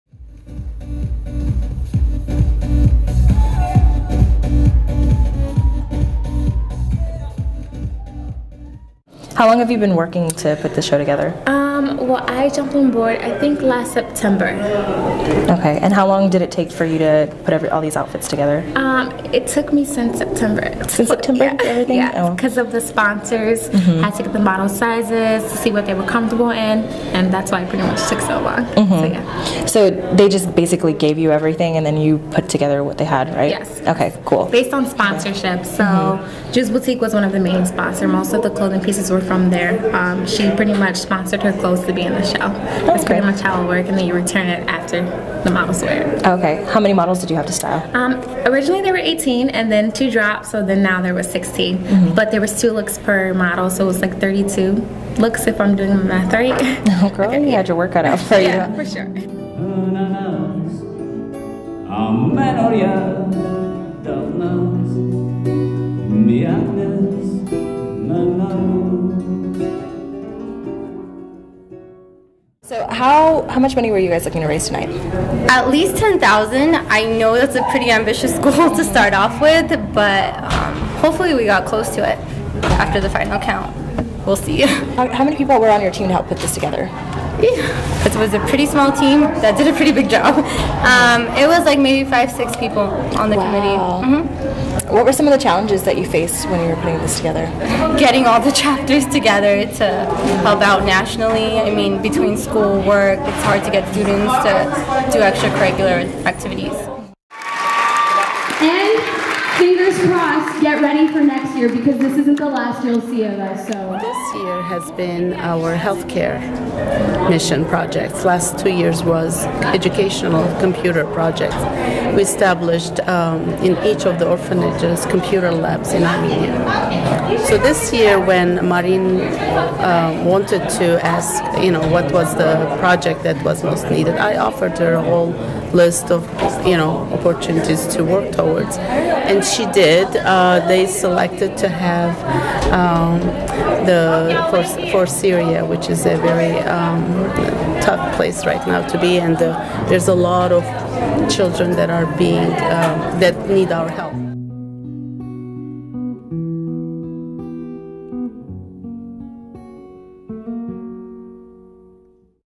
CSUN sorority fashion show raises money for orphans
The sisters of Alpha Gamma Alpha sorority collaborated with the Society for Orphaned Armenian Relief (SOAR) Los Angeles chapter to present "Imagine: Runway to Relief," a fundraiser at the CSUN on Feb. 9.